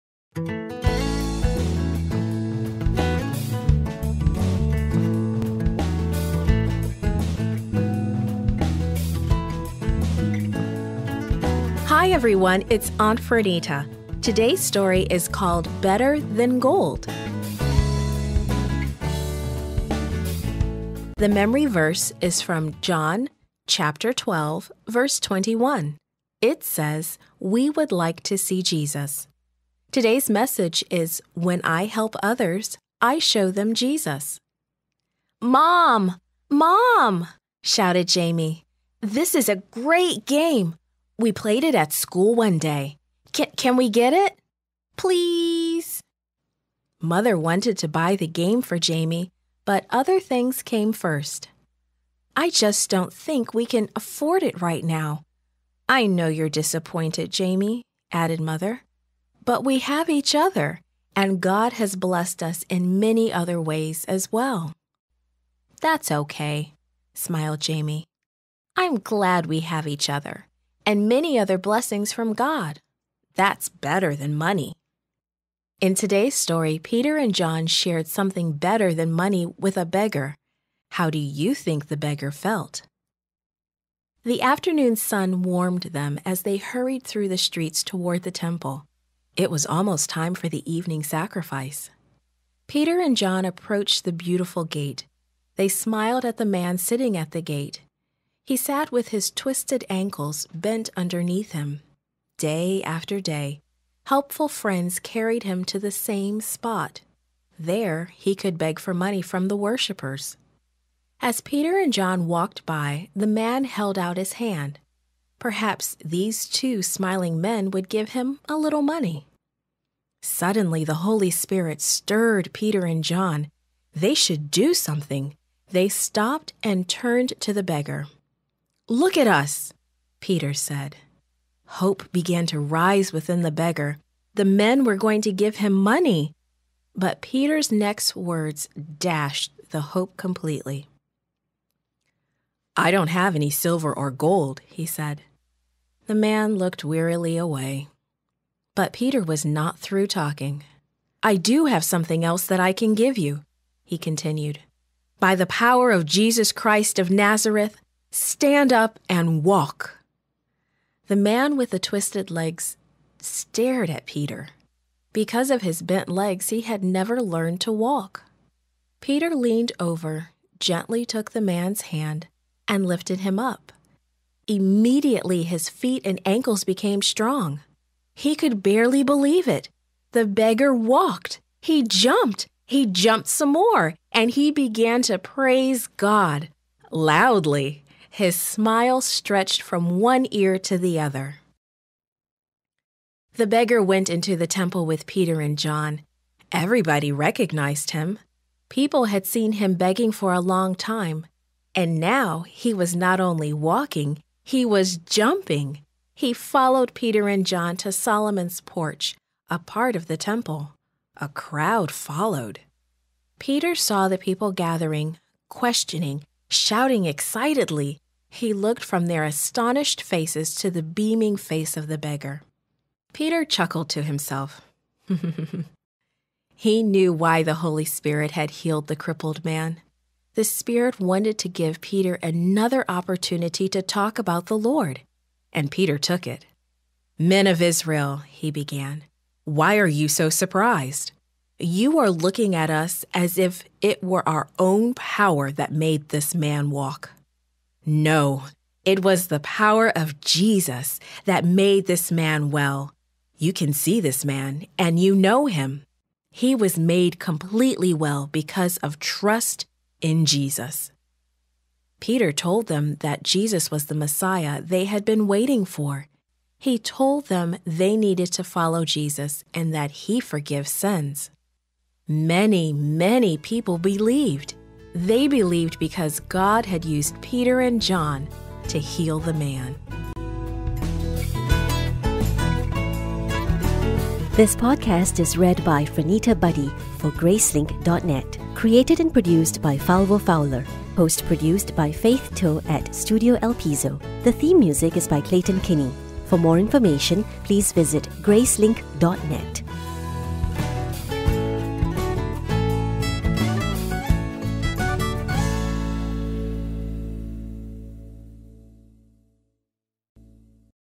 Primary Sabbath School Lesson 6 - Better Than Gold!
When I help others, I show them Jesus.

“ ‘We would like to see Jesus’ ”(John 12:21, NIV).

GraceLink Primary, Year B, Quarter 3. Animated bible stories by gracelink.net